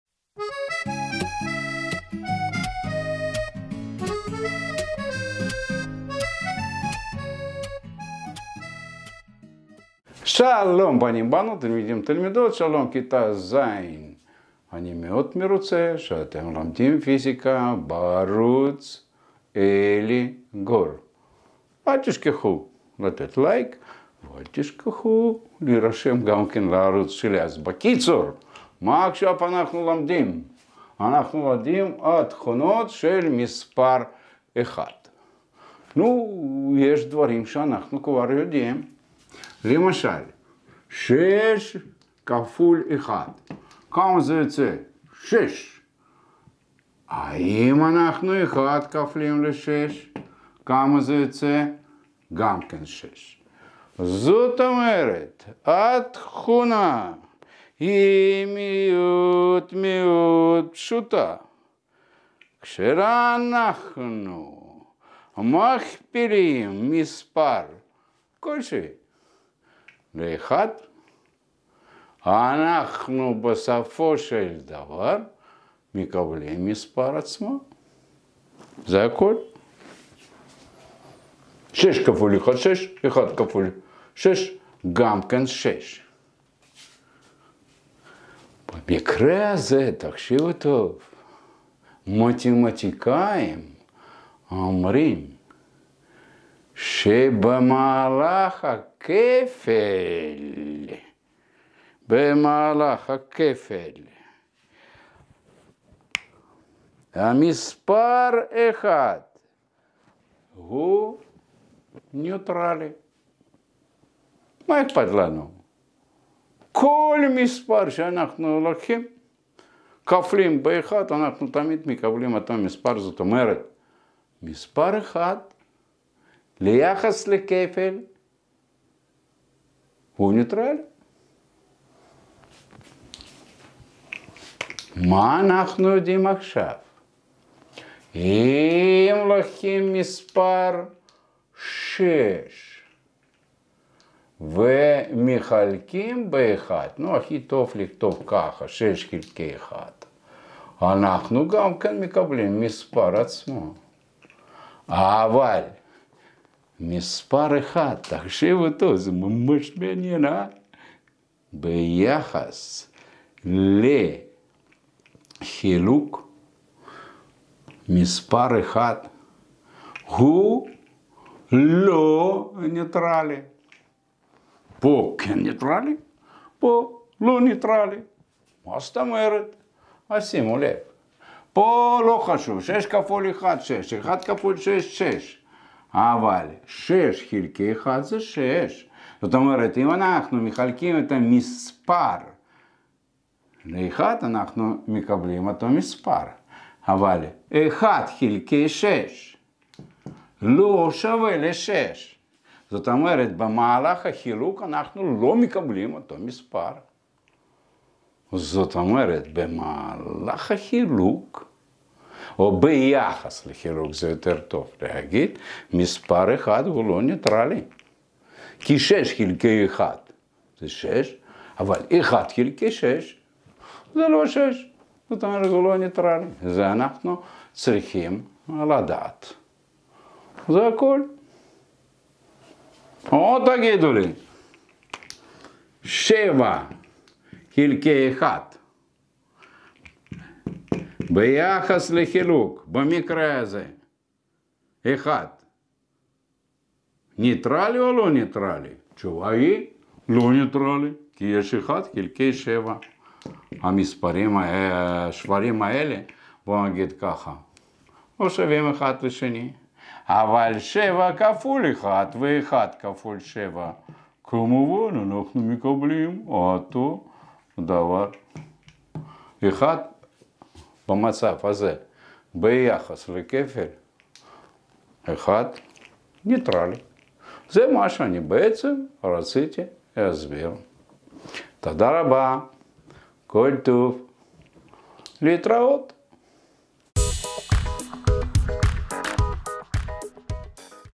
מתמטיקה .כיתה ז . התכונה של מספר 1 MZ9
הסרטון מסביר את התכונות של המספר 1 במתמטיקה, בעיקר במעבר בין כפל וחילוק. הוא מראה ש 1 הוא ניטרלי בכפל, מכיוון שכפל כל מספר ב 1 תמיד נותן את אותו המספר. לעומת זאת, חילוק מספר ב 1 לא תמיד נותן את אותו המספר.